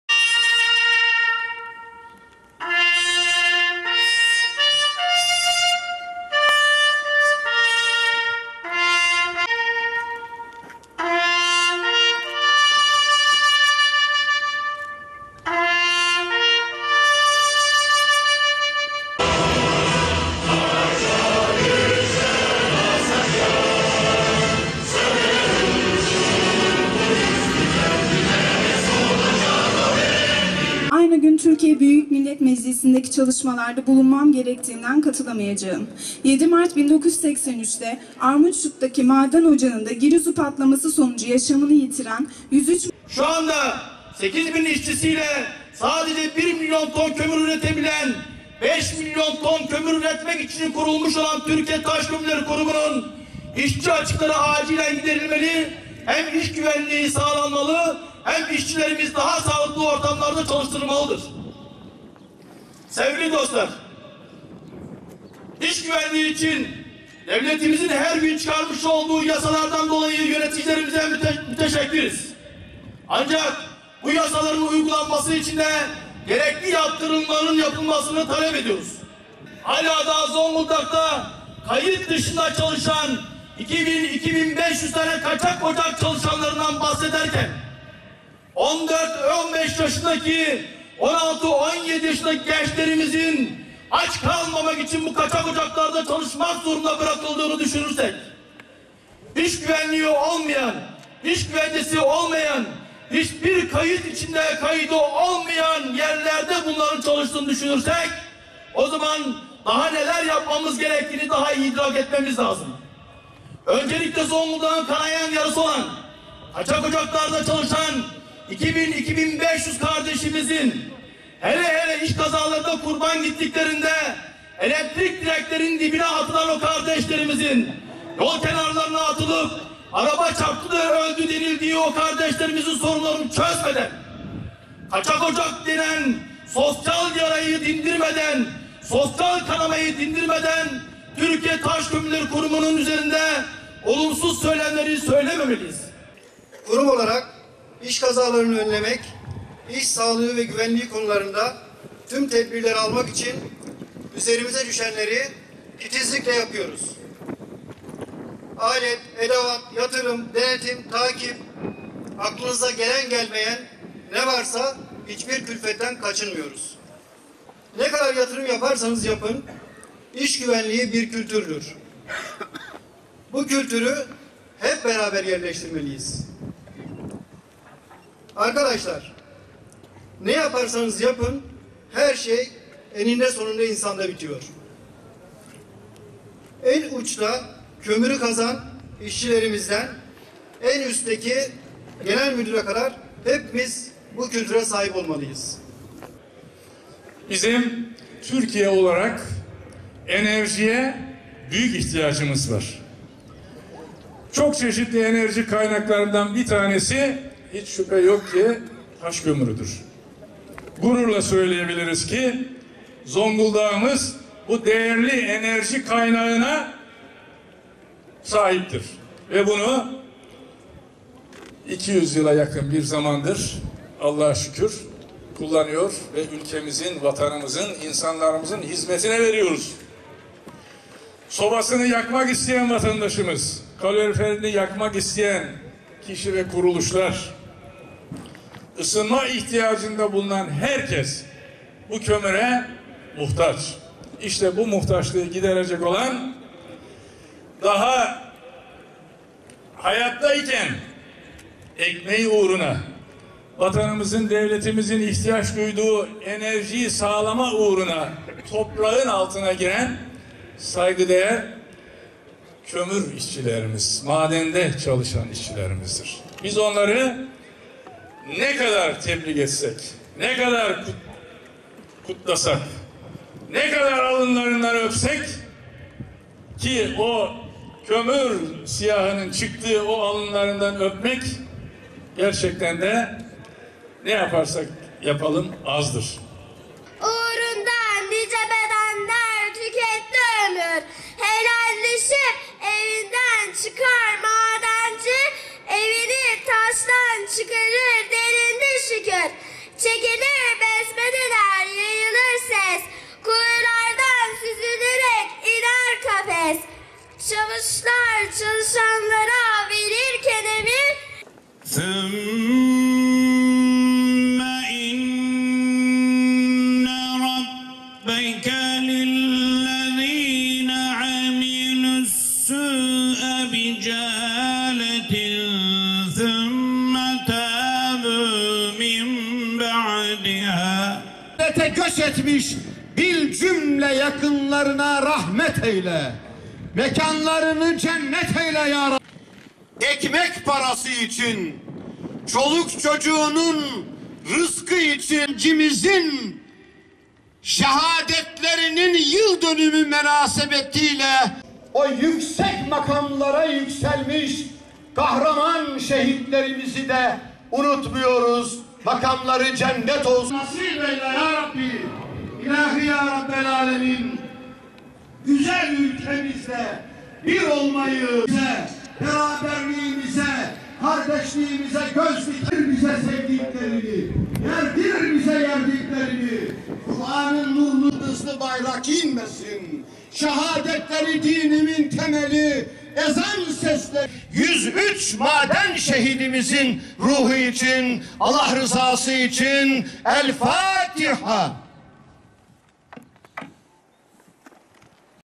103 Madenci dualarla anıldı
Zonguldak'ın Ereğli ilçesine Bağlı Kandilli Beldesi Türkiye Taş Kömürü Kurumu (TTK) Armutçuk müessesinde 7 Mart 1983 yılında meydana gelen grizu faciasında hayatını kaybeden 103 madenci dualarla anıldı.

34 sene önce Türkiye Taş Kömürü kurumu (TTK) Armutçuk Müessesinde meydana gelen grizu faciasında hayatını kaybeden 103 madenci için Armutçuk müessesinde anma programı düzenlendi. Törene İşten çıkan ve işe girmek için hazır bulunan madencilerin yanı sıra Ereğli Kaymakamı Nazım Madenoğlu, belediye başkanları, siyasi parti başkanları, kurum yetkilileri, sendika temsilcileri siyasiler ve sivil toplum kuruluşları katıldı. Tören de saygı duruşunun ardından konuşan Genel Maden İşçileri Sendikası (GMİS) Genel başkan yardımcısı İsa Mutlu, iş ve işçi güvenliği ile ilgili çıkartılan yasaların uygulanması konusunda yaptırım yapılması gerektiğini söyledi. Kaçak ocaklarda ölen işçilerin elektrik direkleri diplerine bırakıldığını hatırlatan Mutlu konuşmasında "Sadece 8 bin işçisi ile 1 milyon ton kömür üretebilen 5 milyon ton kömür üretmek için kurulmuş olan Türkiye Taş Kömürü Kurumunun işçi açıkları acilen giderilmeli, iş güvenliği sağlanmalı, işçilerimiz daha güvenli ortamlarda çalıştırılmalıdır. Devlerimizin iş güvenliği konusunda çıkartmış olduğu yasalardan dolayı müteşekkiriz.  Ancak bu yasaların uygulanması için de gerekli yaptırımların yapılmasını talep ediyoruz. Hala daha Zonguldak'ta kayıt dışında çalışan 2500 kaçak ocak çalışanlarından bahsederken, 14-15 yaşındaki gençlerimizin aç kalmamak için kaçak ocaklarda çalıştırılmak zorunda kaldıklarını düşünürsek. İş ve iş güvencesi olmayan hiç bir kaydı olmayan yerlerde bunların çalıştıklarını düşünürsek daha neler yapmamız gerektiğini daha iyi idrak etmemiz lazım. Öncelikle Zonguldak'ın kanayan yarası olan kaçak ocaklarda çalışan 2 bin 500 kardeşimizin hele hele iş kazalarına kurban gittiklerinde elektrik dibine atılan o kardeşlerimizin, yol kenarına atılıp araba çarptı da öldü denilen o kardeşlerimizin sorunlarını çözmeden kaçak ocak denilen sosyal yarayı dindirmeden Türkiye Taş Kömürü Kurumunun üzerinde olumsuz söylemleri söylememeliyiz" dedi.

Türkiye Taş Kömürü Kurumu Genel Müdürü Ercan Gebeş ,iş güvenliği ile ilgili olarak tüm yatırımların yapıldığını ancak iş güvenliğinin bir kültür olduğunu, işçiden müdüre kadar herkesin bu kültüre sajhip olması gerektiğini söyledi. Gebeş konuşmasında "Kurum olarak iş kazalarını önlemek İş sağlığı ve güvenliği konularında tüm tedbirleri almak için üzerimize düşenleri titizlikle yapıyoruz. Alet edevat yatırım eleman aklınıza gelen gelmeyen ne varsa hiç bir külfetten kaçınmıyoruz. Ne kadar yatırım yaparsanız yapın İş güvenliği bir kültürdür. Bu kültürü hep beraber yerleştirmeliyiz. Ne yaparsanız yapın,  her şey eninde sonunda insanda bitiyor. en uçta kömürü kazan içişlerimizden en üstteki genel müdüre kadar hepimiz bu kültüre sahip olmalıyız" dedi.

Tören de son konuşmayı yapan Ereğli Kaymakamı Nazım Madenoğlu ise konuşmasında" Bizim Türkiye olarak enerjiye büyük ihtiyacımız var. Çok çeşitli enerji kaynaklarından bir tanesi hiç şüphe yok ki Taş kömürüdür. Gururla söyleyebiliriz ki Zonguldak'ımız bu değerli enerji kaynağına sahiptir. VE bu kömürü 200 yıldır çıkartıp insanımızın ülkemizin hizmetine veriyoruz. Sobasını, kaloriferini yakmak isteyen enerji üretmek isteyen herkes bu kömüre muhtaç. İşte bu muhtaçlığı giderecek olan daha hayattayken ekmeği uğruna vatanımızın devletimizin ihtiyaç duyduğu enerjiyi sağlamak uğruna toprağın altına giren saygı değer kömür, maden işçilerimizdir. Biz onları Biz onları ne kadar tebrik etsek ne kadar kutlasak ne kadar alınlarından öpsek azdır" dedi.

Konuşmaların ardından Armutçuk Madenci İlkokul öğrencisi Ayça Uzun 'Madenci' şiirini okudu.

Yapılan konuşmaların ardından 7 Mart 1983 yılında grizu faciası başta olmak üzere hayatını kaybeden madenciler için Kur'an-ı Kerim okundu ve mevlit şekeri dağıtıldı.